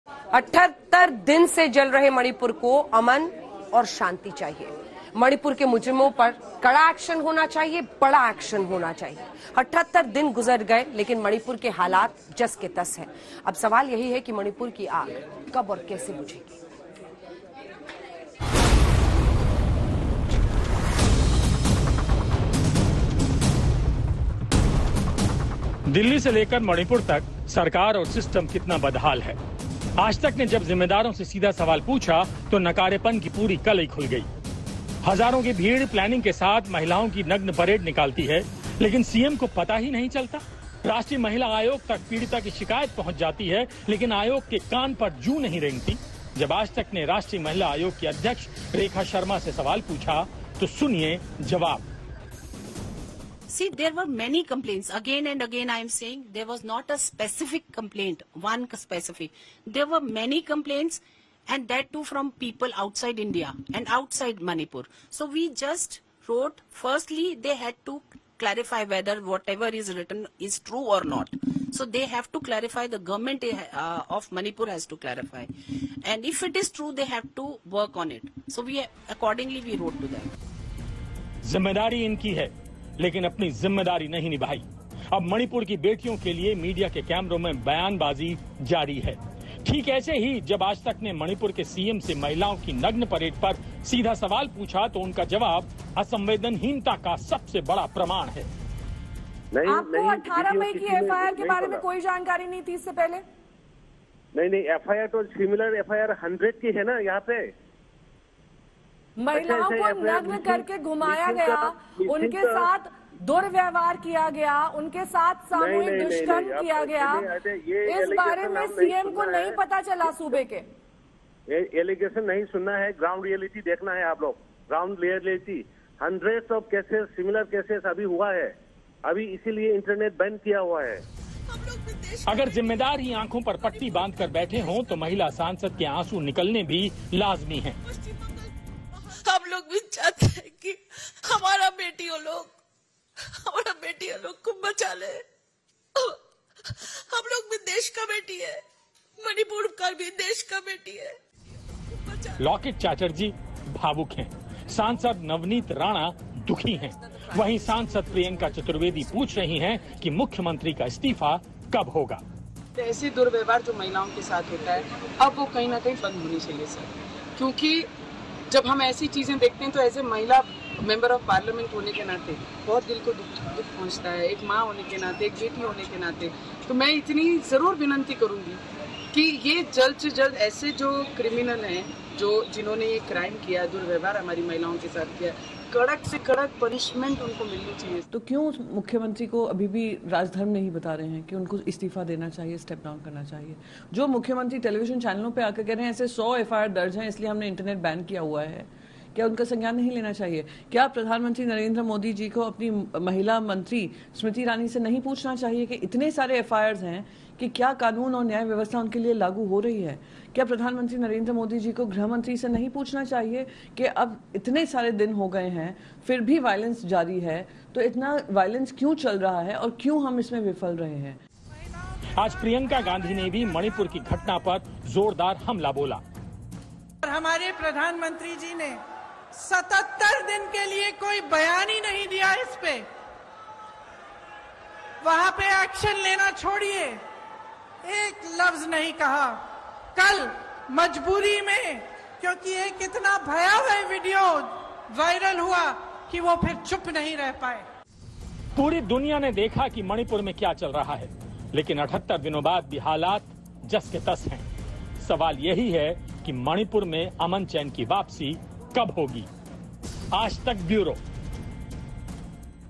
Special Report: Manipur Violence पर AajTak के सवाल पर हिल गई N Biren Singh सरकार | BJP
महिलाओं से हैवानियत को लेकर मणिपुर में चार लोग पकडे जा चुके हैं और बाकियों की तलाश जारी हैः लोगों का गुस्सा इस कदर है कि मुख्य आरोपी का घर तक फूंक दिया गया और लोग विरोध में सडकों पर उतर आए । सरकार ने 12 स्पेशल टीमें बना दी है और 12 आरोपियों की धरपकड के लिए ऑपरेशन तेज कर दिया गया है.